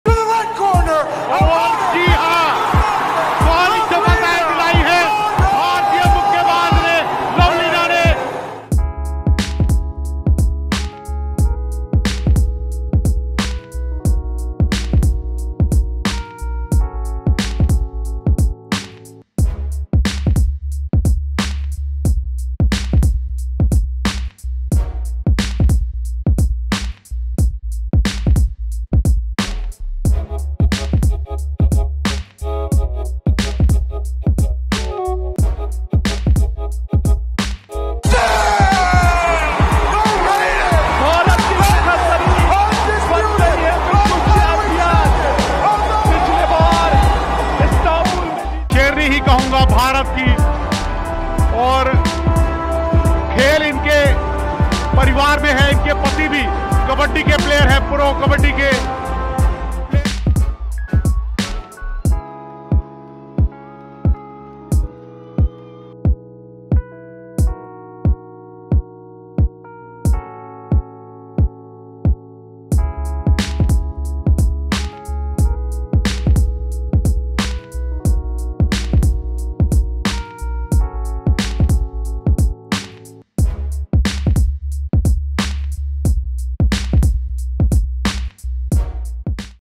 India Shines Bright At World Boxing Championships | G Plus
India shone bright at the Women's World Boxing Championships and won four gold medals. Team India also bagged the Best Team Award.